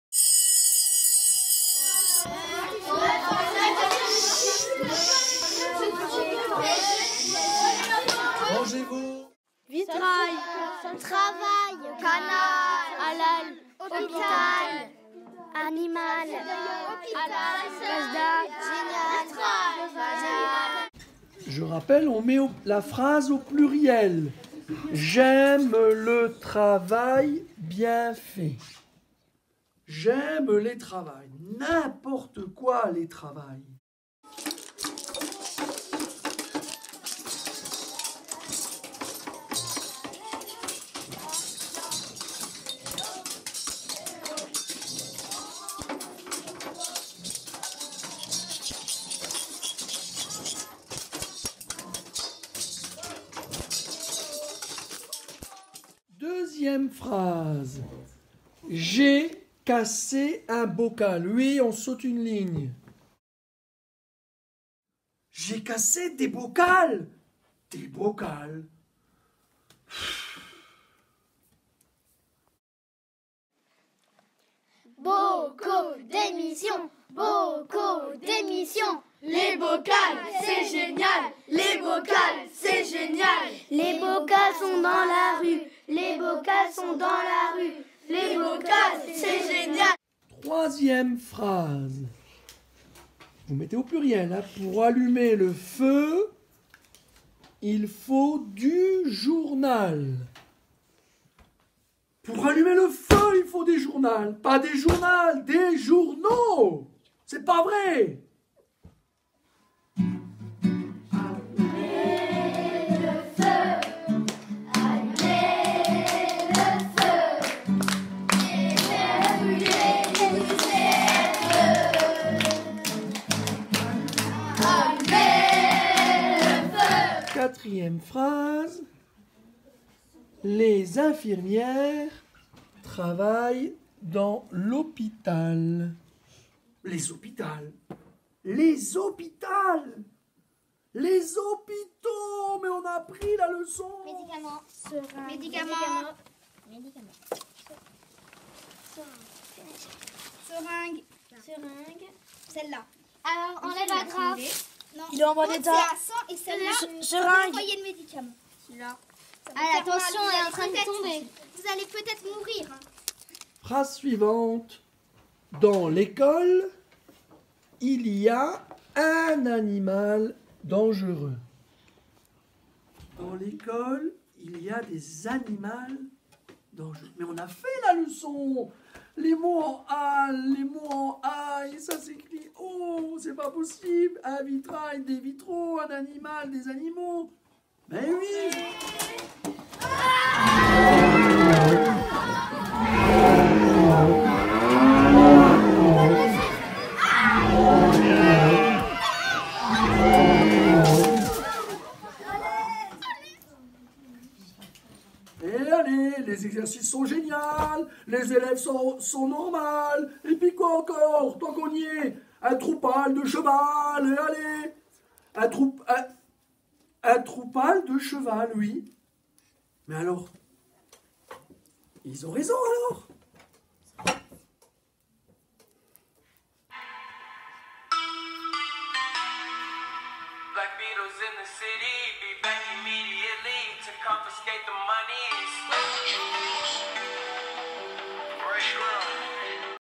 Mosellire 2019 Ecole Lenotre de Richemont Cat CM1 CM2 3e prix partenariat
Dans le cadre du concours départemental des jeunes lecteurs MoseL’lire 2018/2019, les participants pouvaient présenter un livre de la sélection sous la forme d’une vidéo, à la manière de booktubeurs.

Parmi les productions vidéos lauréates de cette 12e édition, voici le booktube de l'Ecole Lenotre de Richemont Cat  CM1 CM2   3e prix partenariat.